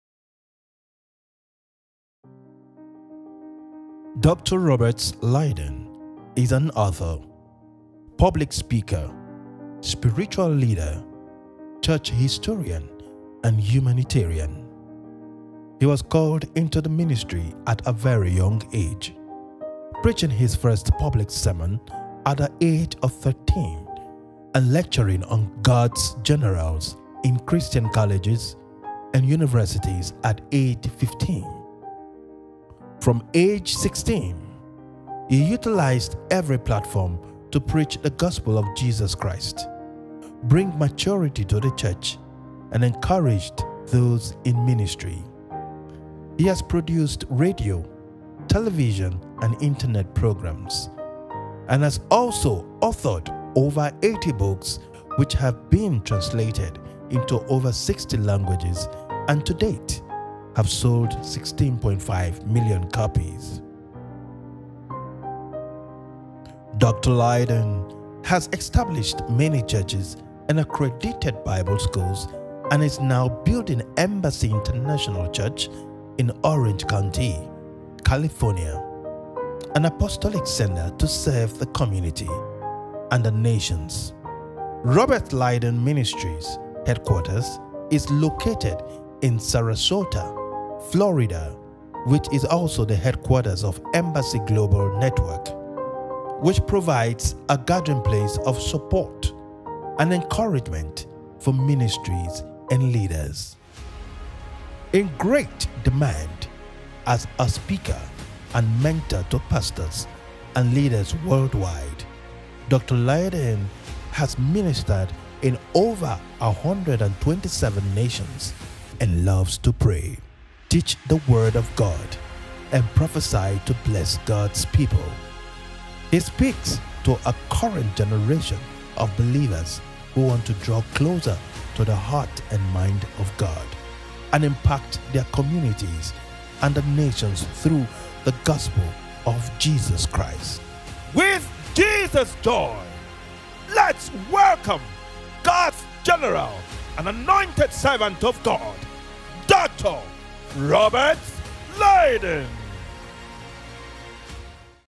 ROBERTS LIARDON WAS IN OUR CHURCH THIS YEAR 2021
What an honour it was to have this seasoned man of God in Nigeria. Watch his biography and enjoy!